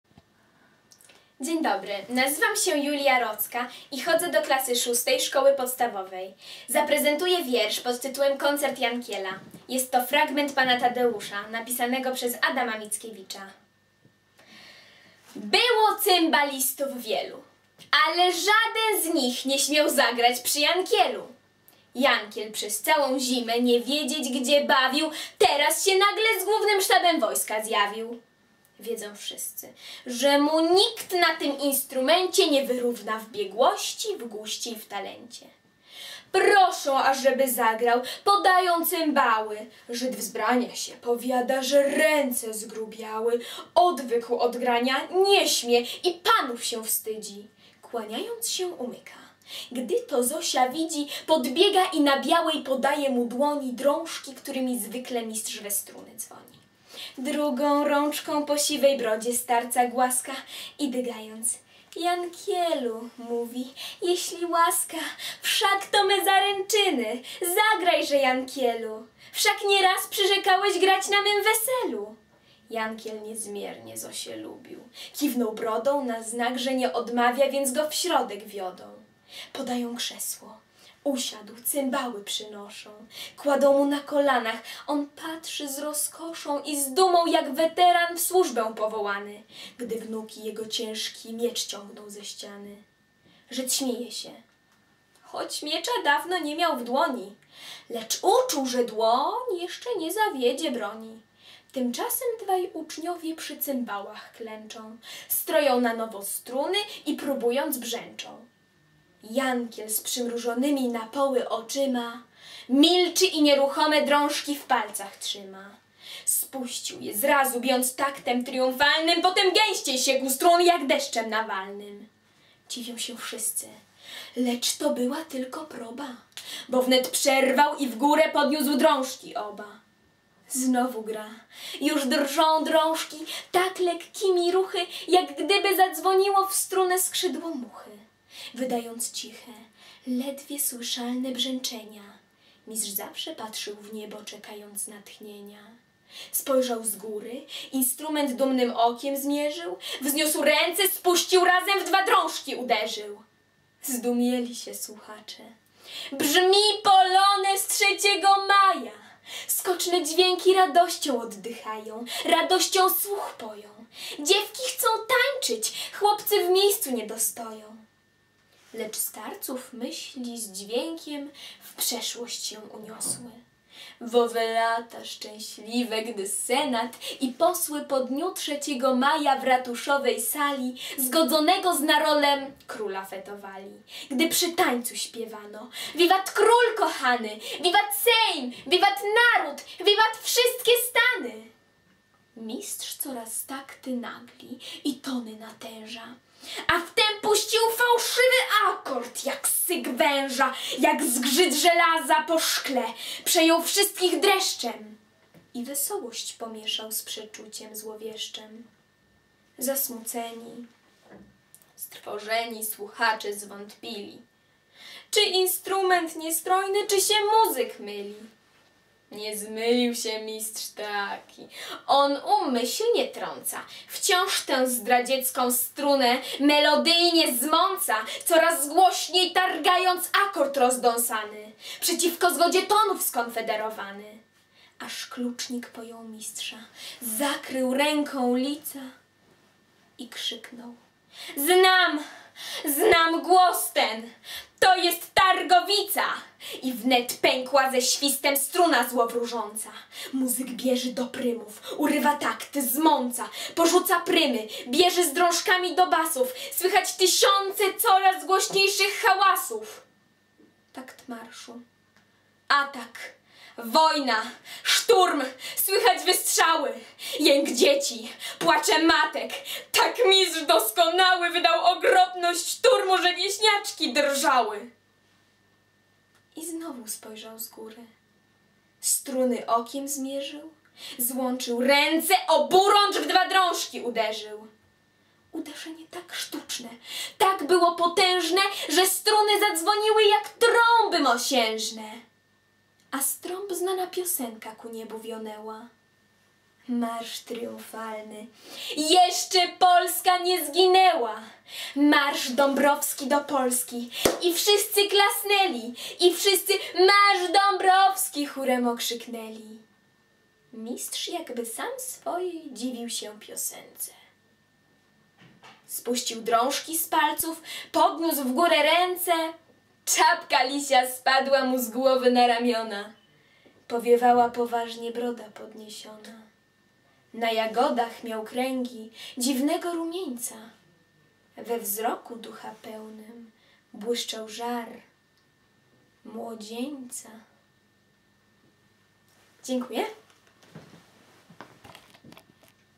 01883.MTS
I miejsce w konkursie "Witaj Majowa Jutrzenko"
Koncert Jankiela - rec. Julia Rocka
marzec 2012